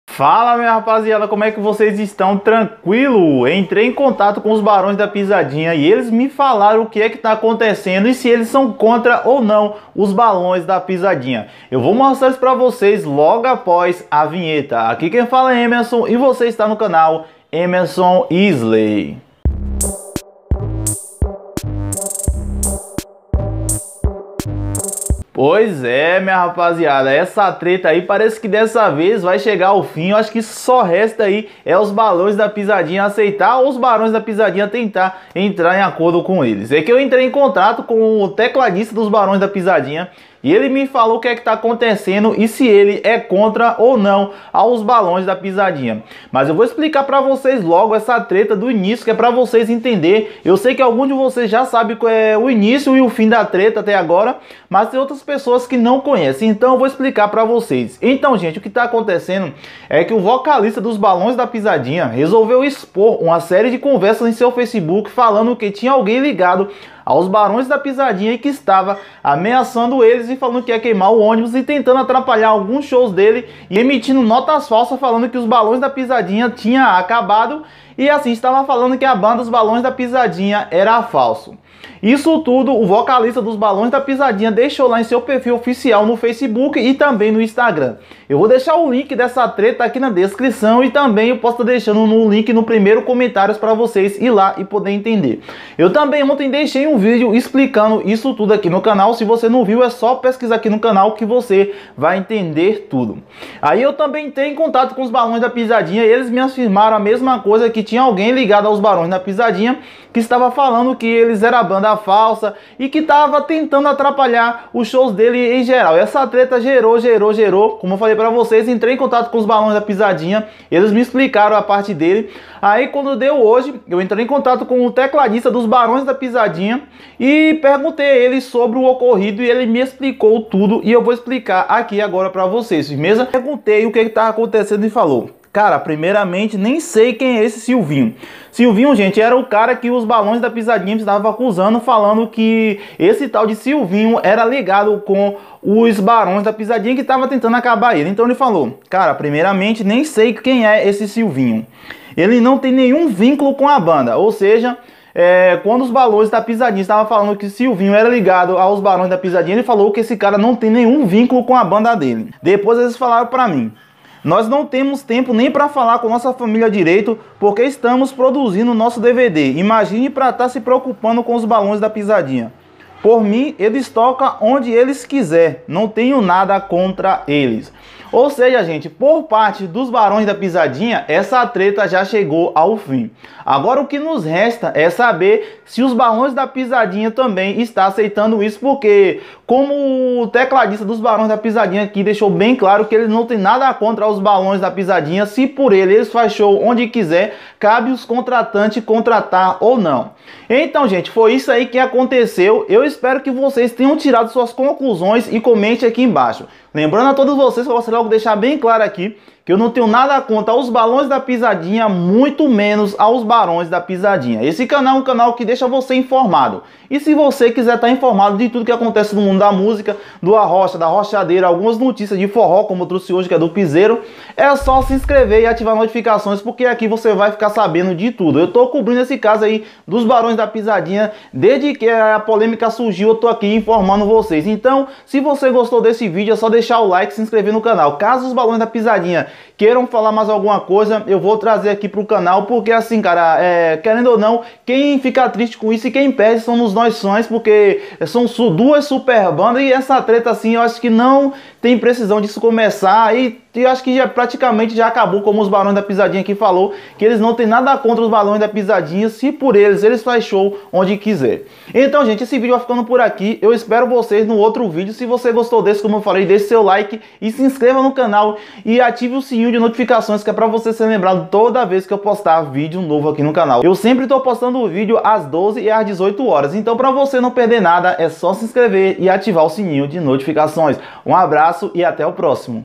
Os barões da pisadinha falou sobre a polêmica envolvendo os balões da pisadinha!
Poes e rapaziada os barões da pisadinha se pronunciou sobre a p@lemica envolvendo os balões da pisadinha!

Quer saber de todos os lançamentos,e saber antes  mesmo do vídeo ser postado aqui no YouTube?